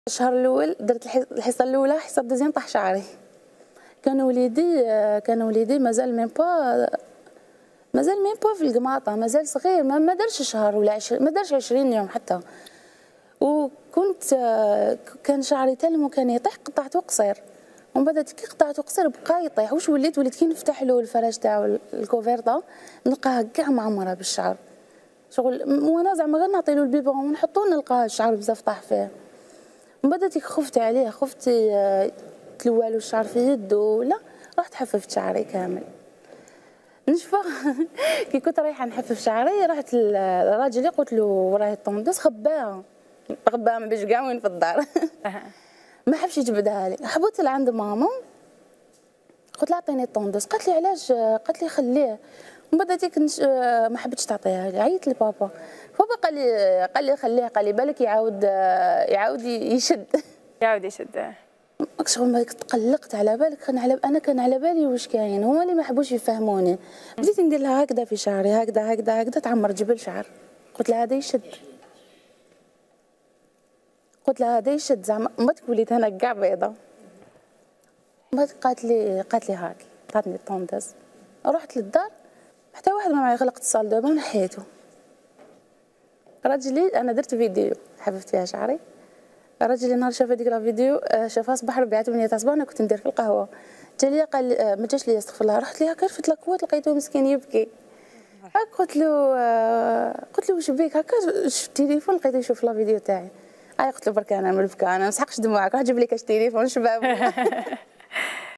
الأنستغراموز أمينة تروي قصة حلق شعرها "بالزيرو" وهذا هو السبب الصادم وراء ذلك.. شاهدوا
من برنامج